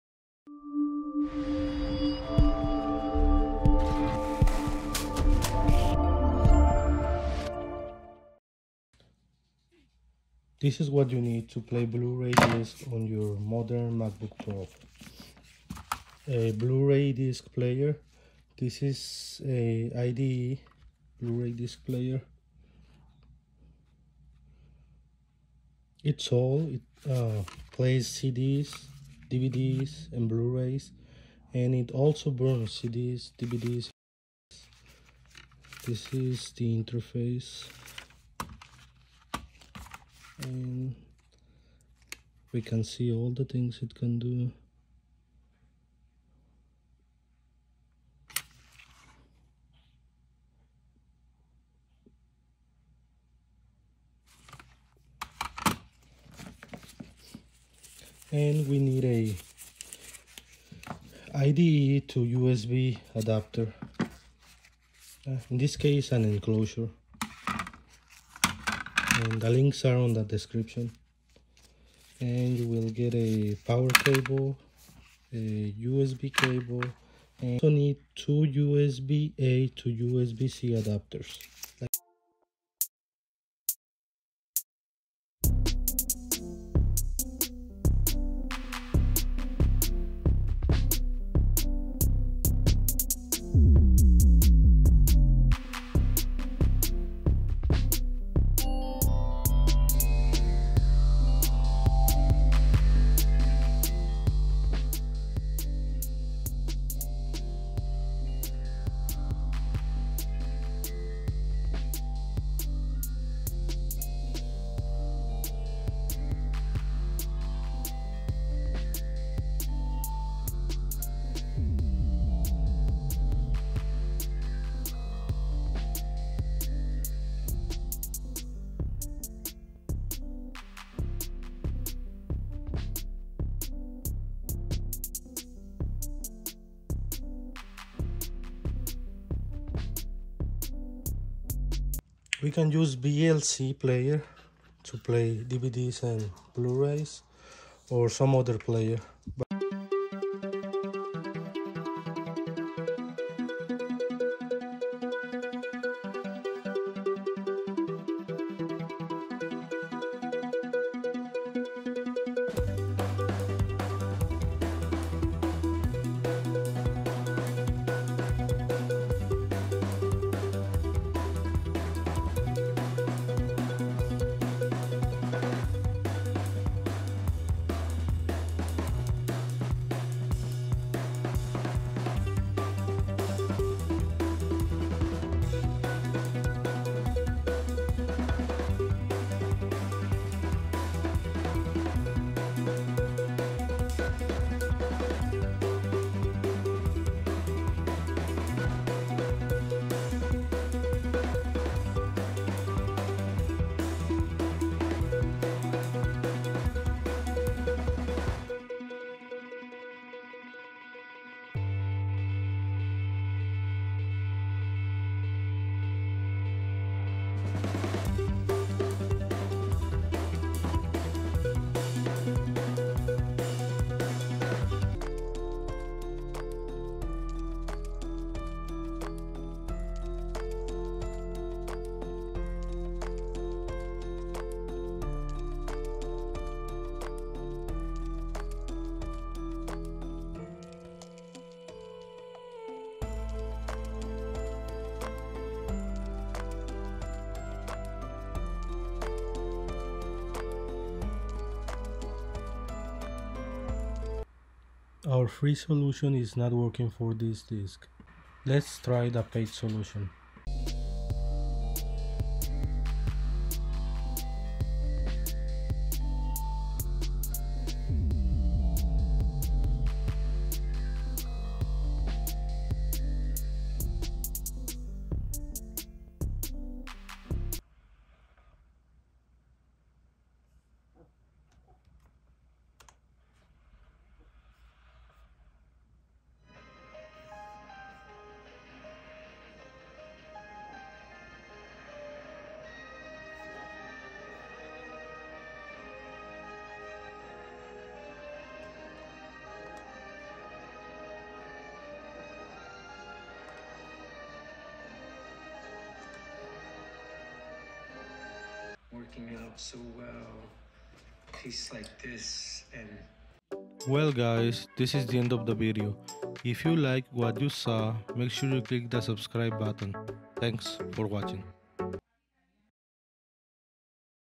Macbook MacOS | how to play blu-ray discs on a modern MacBook
USB to ide enclosure:

Blu-ray IDE player, Sony BC-5500A, there are many others that work...